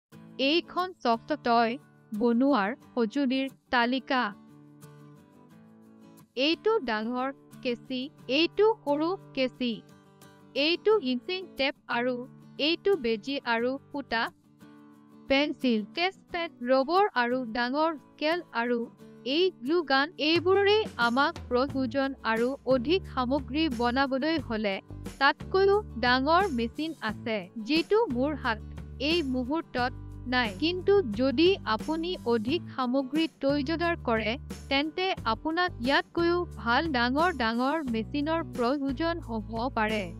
Normal Sewing Machine_Soft Toy Maker & Mat Weaver _Assamese
Usage of Normal Sewing Machine for a Soft Toy Maker & Mat Weaver  explained in Assamese